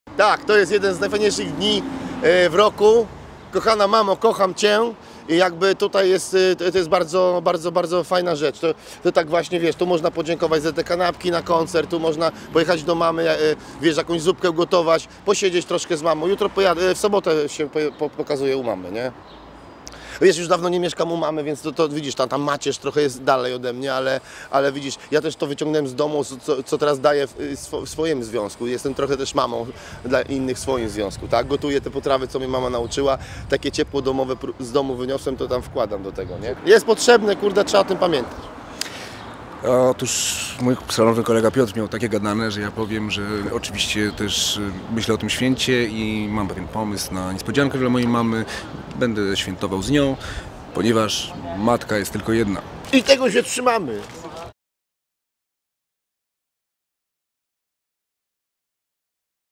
Vienio i Garlicki o Dniu Matki
Panowie Więcławski i Garlicki przypominają o Dniu Matki!